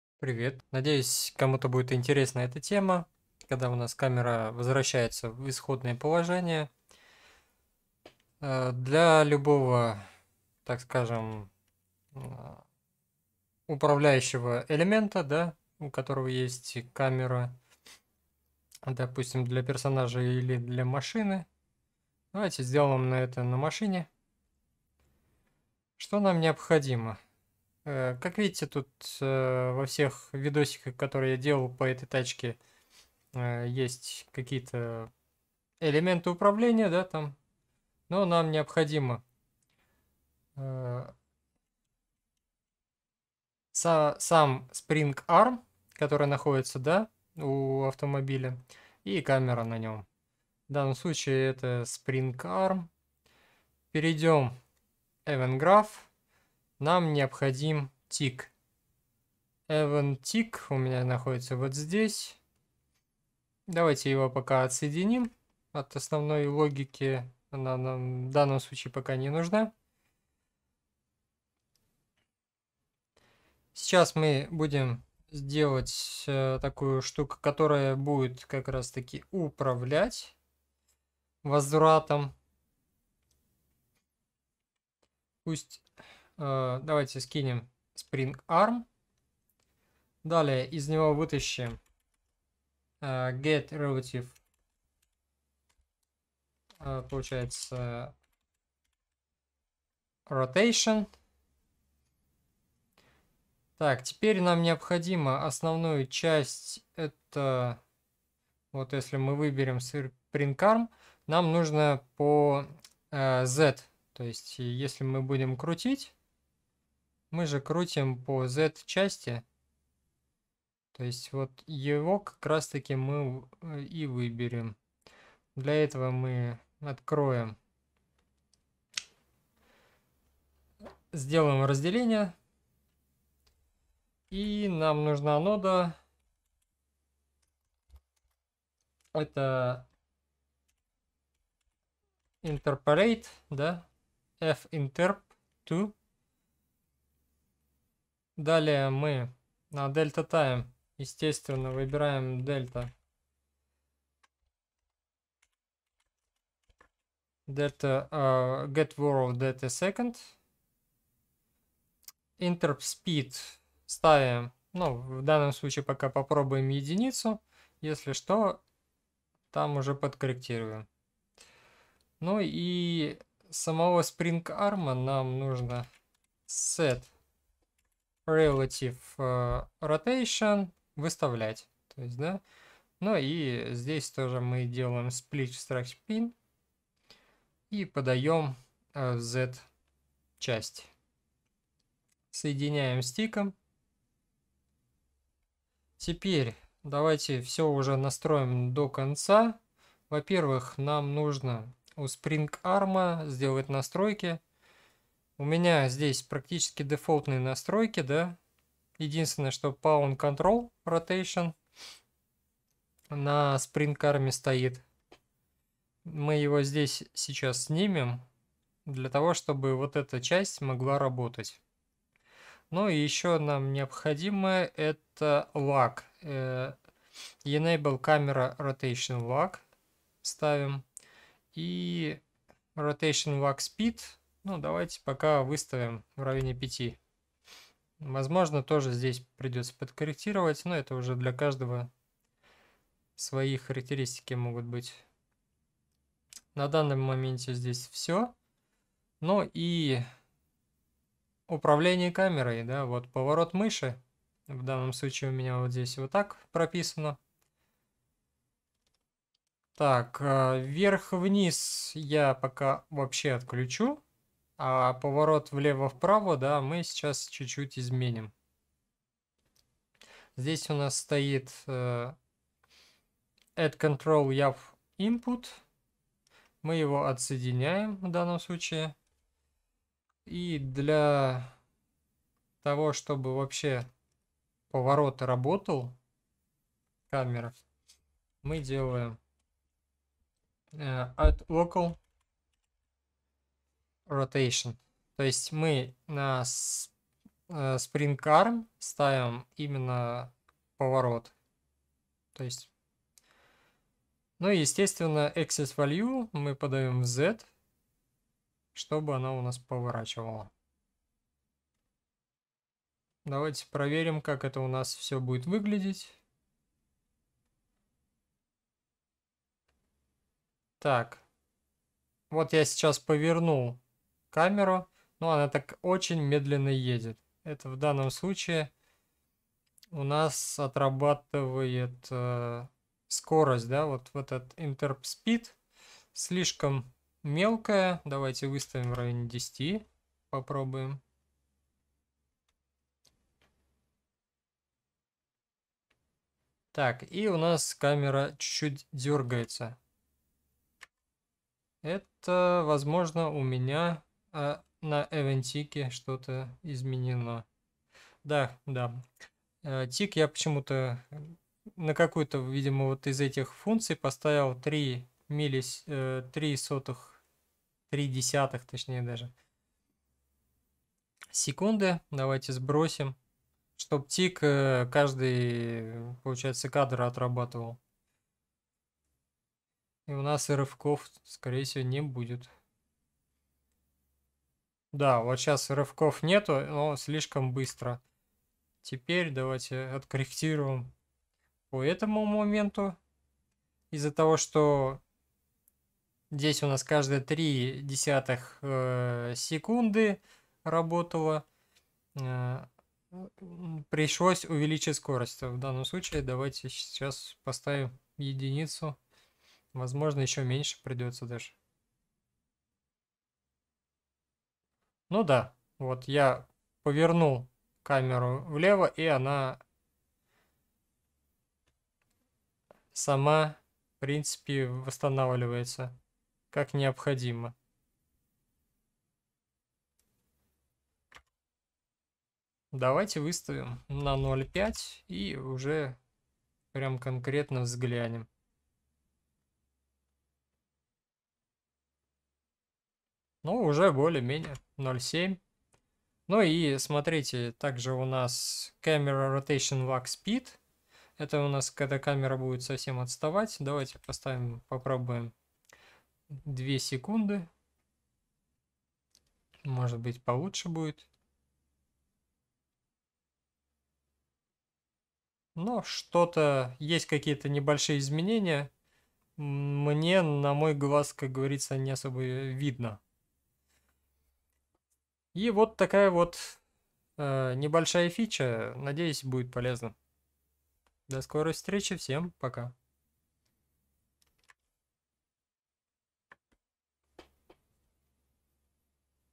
Возврат камеры в начальную позицию (UE4, UE5)
Сделаем несложный возврат камеры в начальную позицию при повороте ее вокруг персонажа